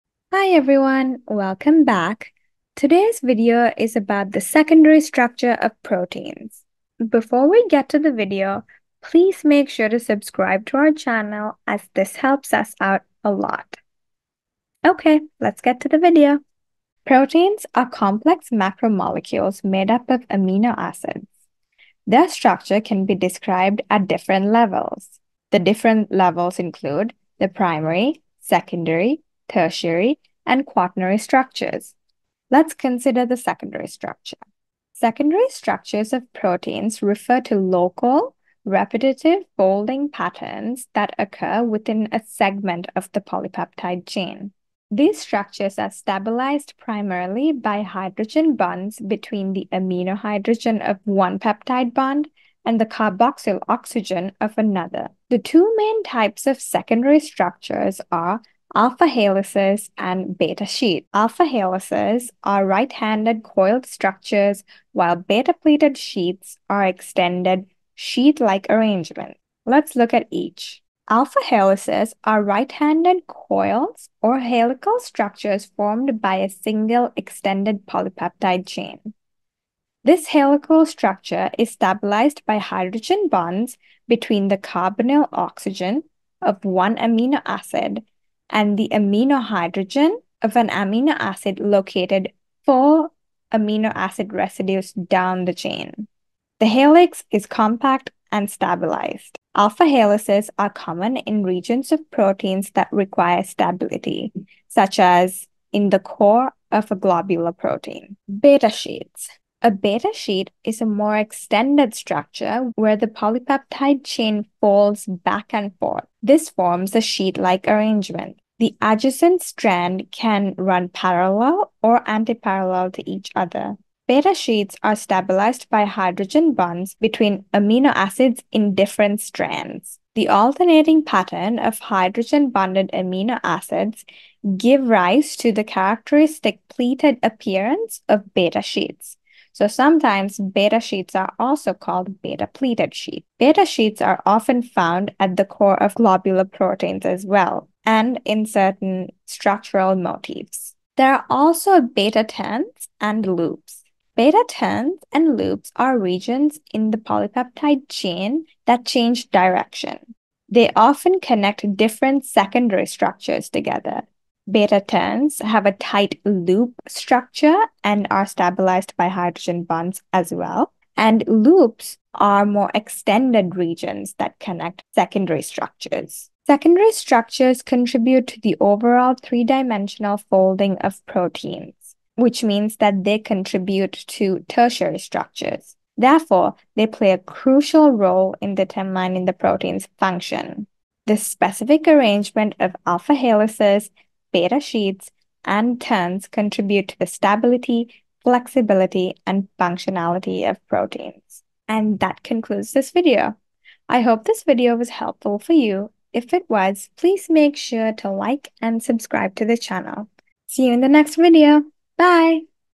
Secondary Structure of Proteins | MCAT Biochemistry
This video talks about the secondary structure of proteins. The secondary structure of proteins refers to the local spatial arrangements of the polypeptide chain. It is primarily characterized by regular, repeating structural patterns that result from hydrogen bonding between amino acids. The two main types of secondary structures are the alpha helix and the beta sheet.

Alpha Helix:
In an alpha helix, the polypeptide chain adopts a helical or spiral shape.
The backbone of the helix is stabilized by hydrogen bonds formed between the carbonyl oxygen of one amino acid and the amino hydrogen of an amino acid located four residues down the chain.
Alpha helices are common in regions of proteins that span cell membranes or in parts that require a compact and stable structure.

Beta Sheet:
In a beta sheet, the polypeptide chain folds back and forth on itself, forming a sheet-like structure. The backbone of the sheet is stabilized by hydrogen bonds formed between adjacent strands of the polypeptide chain. Beta sheets can be parallel (strands run in the same direction) or antiparallel (strands run in opposite directions), influencing the overall stability and structure of the protein.

These secondary structures often coexist within a protein, and their arrangement contributes to the protein's tertiary structure (overall 3D shape). The secondary structure is crucial for understanding a protein's folding and function.

This video will help you learn the basics and foundational knowledge needed to ace your exams!

Timestamps
0:00 Intro
0.17 Intro to protein structures
0:35 Intro to secondary structures of proteins
0:56 Two types of secondary structures
1:11 Alpha helices of secondary protein structures
1:48 Beta sheets of secondary protein structures
2:31 Beta turns and loops
2:56 The impact of secondary structures of tertiary structures of proteins
3:21 Outro

I hope this was helpful for you.

Check Out Some of Our Other Lessons